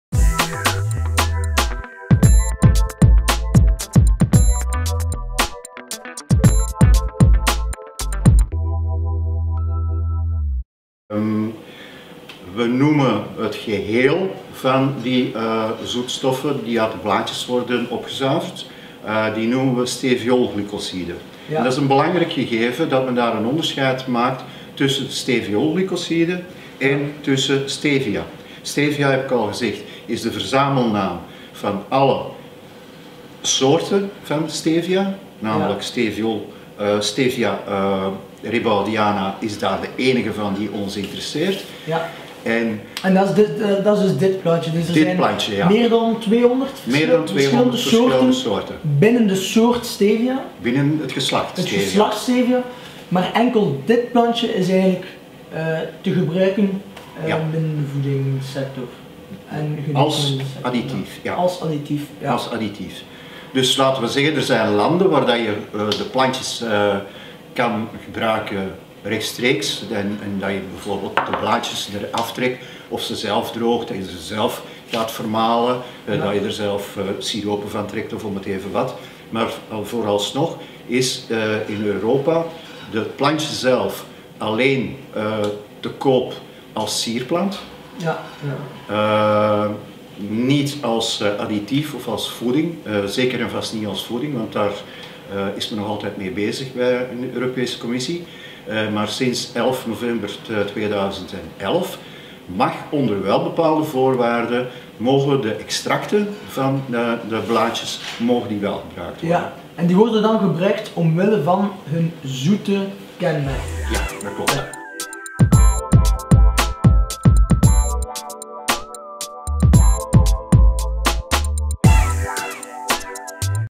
Stevia - wat is stevia?.mov
Leander Verbraeken, personal trainer van TROOP-OUT!, neemt een interview af van Stevia.eu Initiator Roland Lemaire, over het gebruik en de toepassingen van Steviolglycosiden in België.